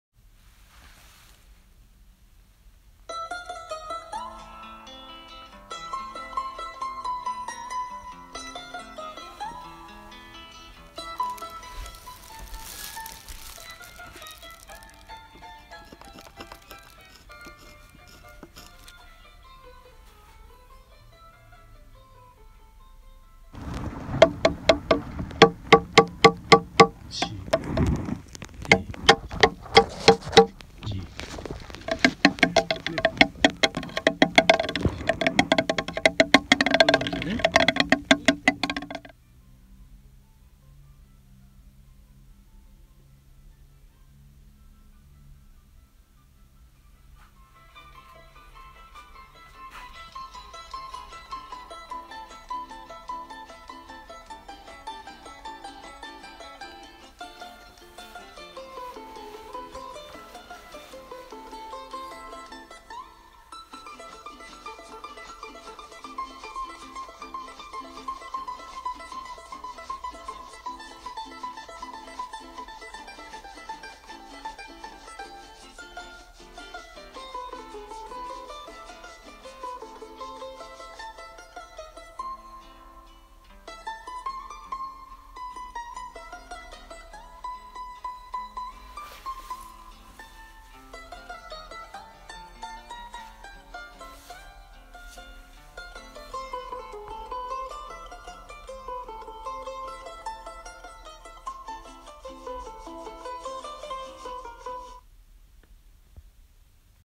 縄文丸木舟5、土器磨きと四角い帆
佐倉市宮内井戸作遺跡の出土注口土器を鯛牙とメノウ棒で磨いて、午後は印旛沼に丸木舟で出ます。木琴となったアウトリガーと四角い帆で風を受けます。